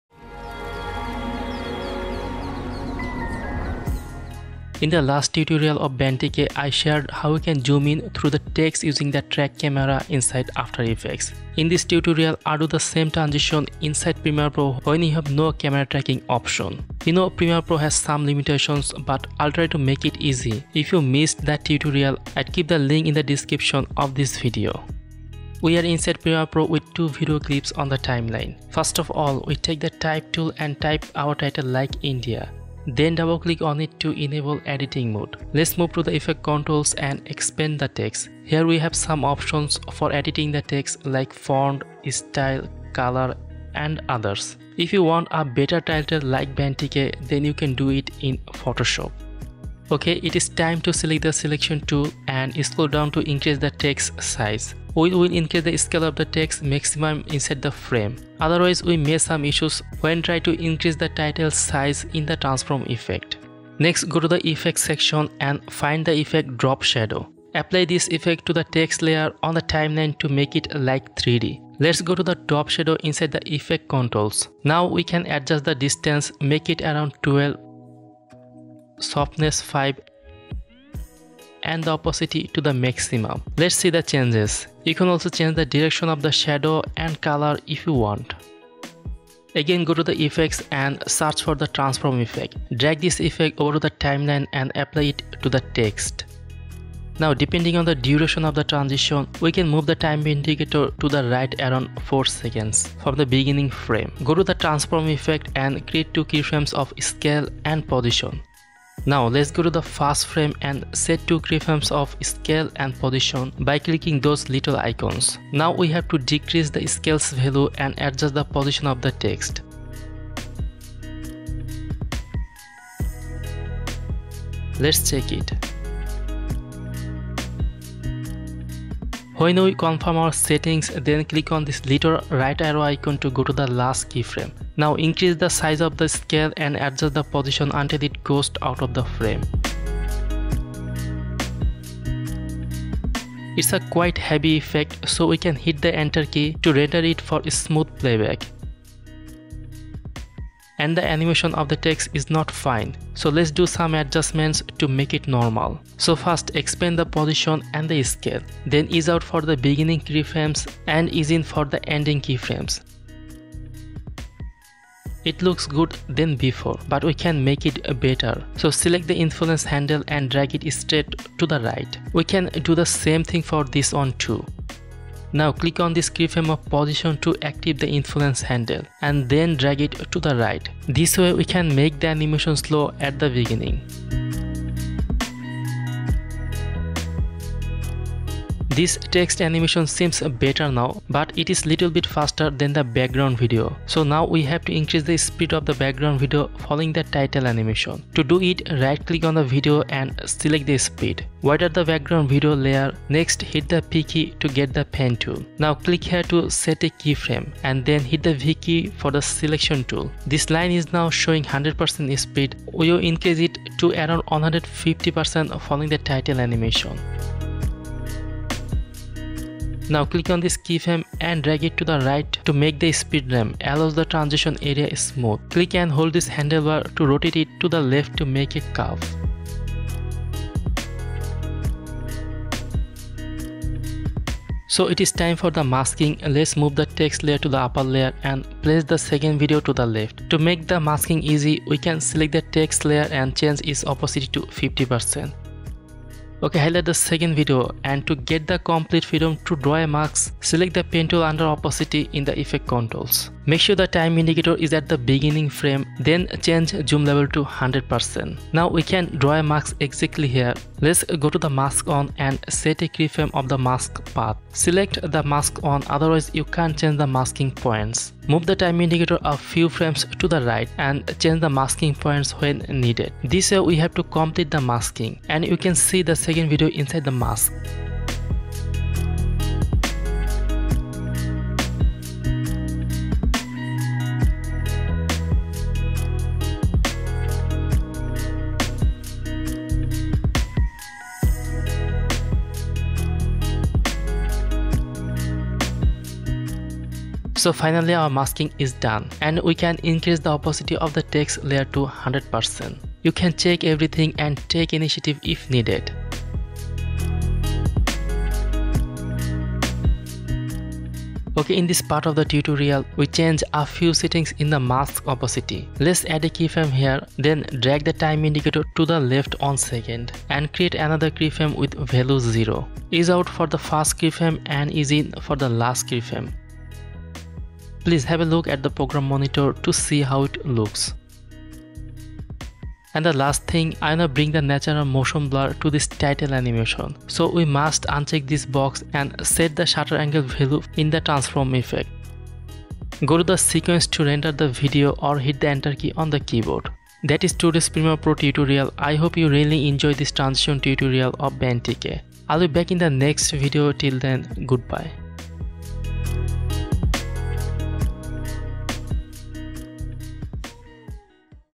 Benn TK Masking Title Zoom in Transition in Premiere Pro
Benn TK's tutorial showcased a text zoom with After Effects' track camera. Learn how to recreate it in Premiere Pro, even without camera tracking: Start with text (e.g., "India") using the type tool. Customize font, style, color in effect controls. Adjust text size within the frame. Apply drop shadow for a 3D effect. Add transform effect from effects panel. Set keyframes for scale and position. Smoothly animate scale and position. Mask with the pen tool for visual depth. Complete masking to reveal a second video layer. Premiere Pro empowers you to replicate this engaging text zoom, inspired by Benn TK's tutorial, without requiring camera tracking. Let’s dive into the After Framing YouTube channel and watch the full tutorial.

⚡ Don’t Miss Out: Creator Savings Roundup